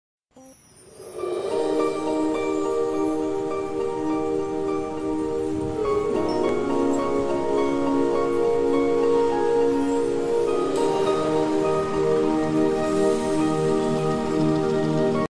A little slideshow of the Harry Potter Studio Tour!
It was an absolutely amazing tour and I will remember these fantastic memories for the rest of my life time. I recommend going to the tour and seeing everything yourselves, don't forget to buy a chocolate frog and get some butterbeer! xXx